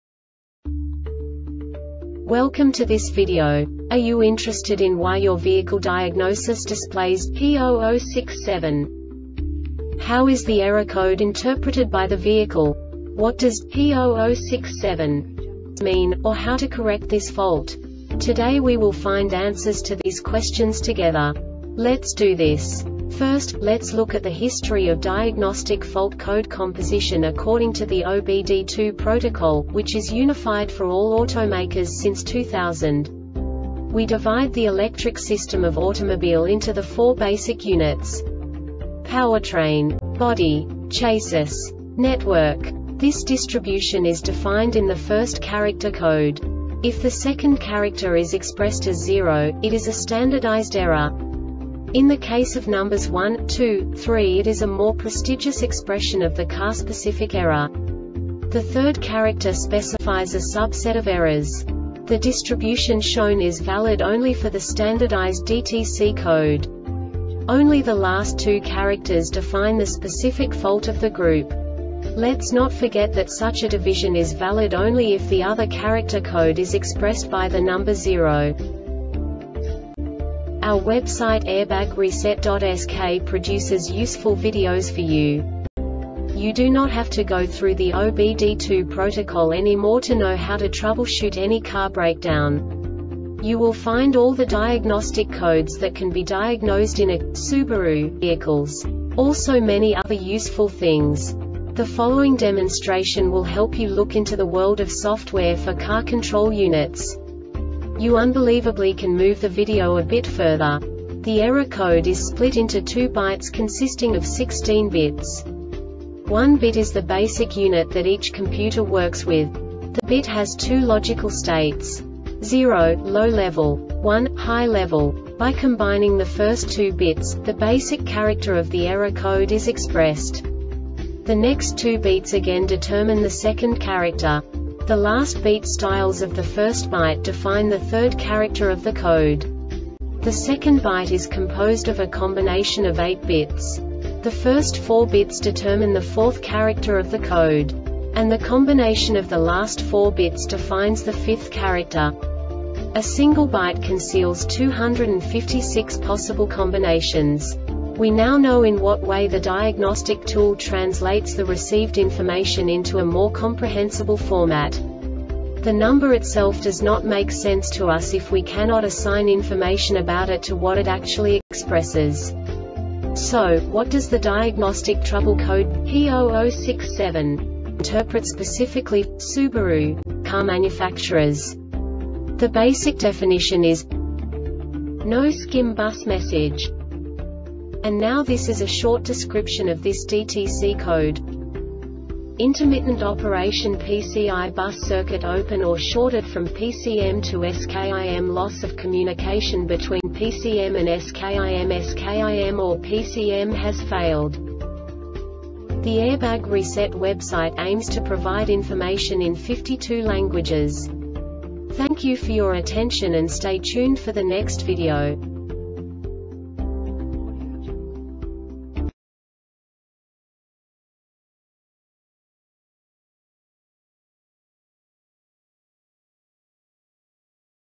DTC Subaru P0067 Short Explanation
Contents:
0:21 Basic DTC analysis according to OBD2 protocol standard.
1:48 Insight into programming
2:58 A diagnostically understandable description of the Diagnostic Trouble Code
3:05 Basic error definition

Make:
Subaru
Code:
P0067
Definition:
No SKIM Bus Message
Cause: